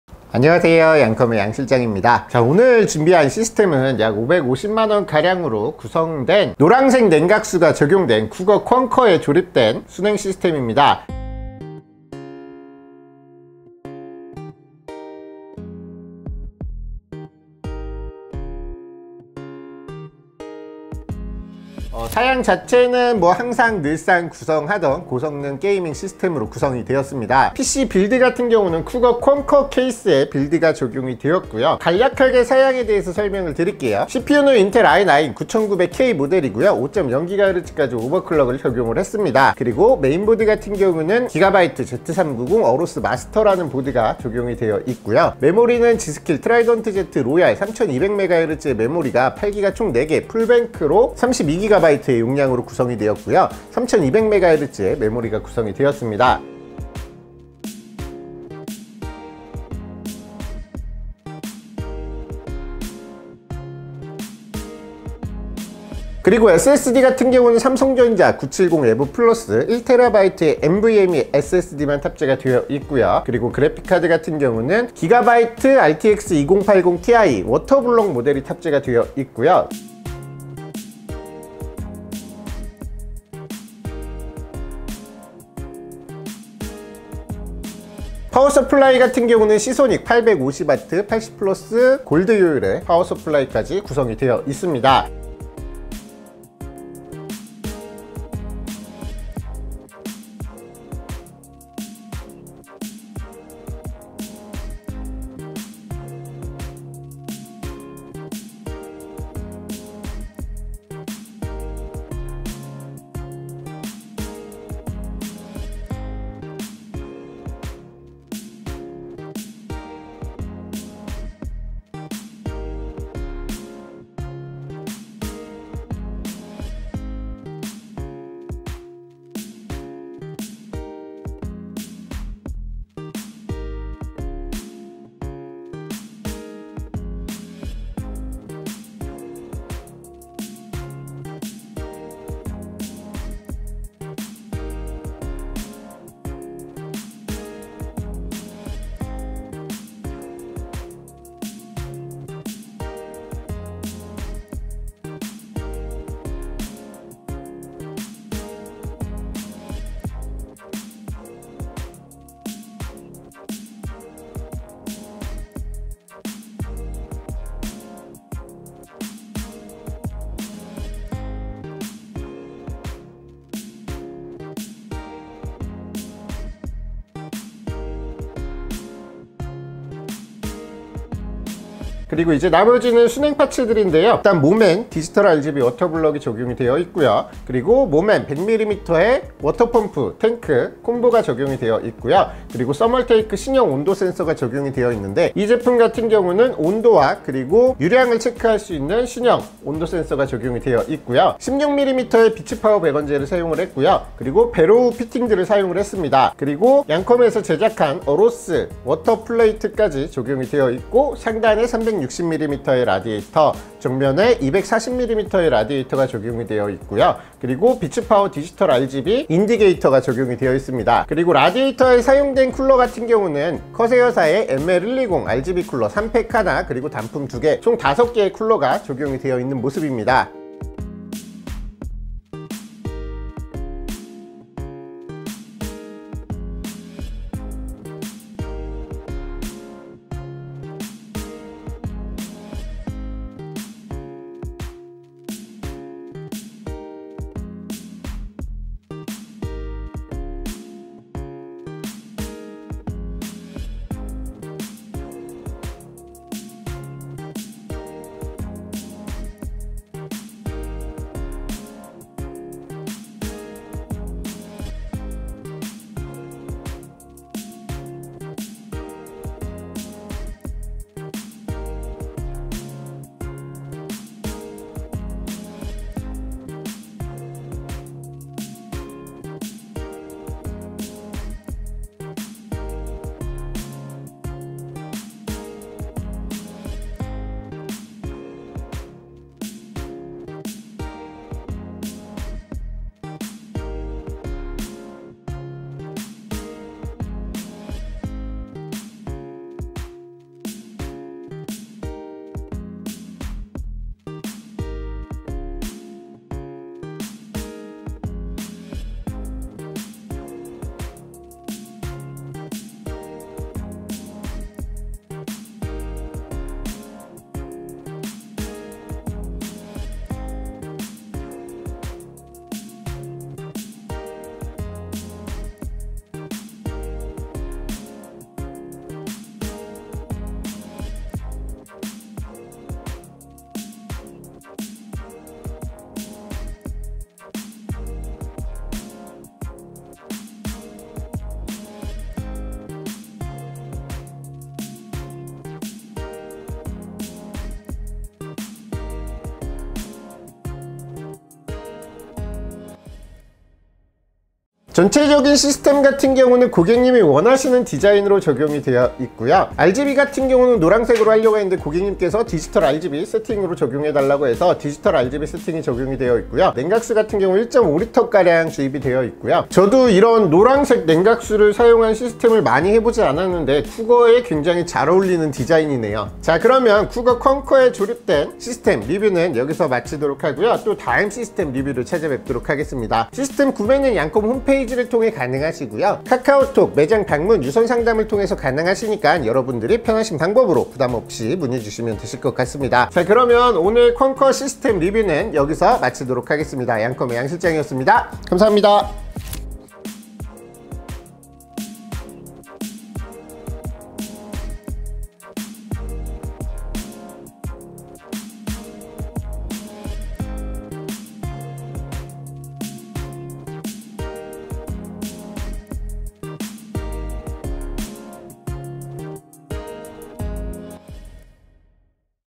노랑노랑 커스텀수냉PC
CPU: 인텔 코어 i9-9세대 9900K
메인보드: GIGABYTE Z390 AORUS MASTER
메모리: G.SKILL DDR4 32G PC4-25600 CL16 RIDENT Z ROYAL 실버 (8GBx4)
SSD: 삼성전자 970 EVO Plus 1TB
그래픽카드: GIGABYTE AORUS XTREME 지포스 RTX2080Ti D6 11GB 워터블럭
케이스: COUGAR CONQUER 강화유리
파워: 시소닉 FOCUS PLUS SSR-850FX GOLD Full Modular
쿨러: CORSAIR ML120 PRO RGB (3Pack), CORSAIR ML120 PRO RGB (1Pack)(2EA)
수냉파츠: 모맨 DRGB CPU WB, 모맨 DRGB 100mm PUMP, Magicool 360mm , Magicool 240mm,
16mm 배관, 비츠파워 DRGB 인디게이터, BARROW 블랙 16mm 피팅류, 써멀테이크 온도센서,
양컴 퀀쿼 전용 어로스 수로판, 압력밸브, 배수밸브, 옐로우 냉각수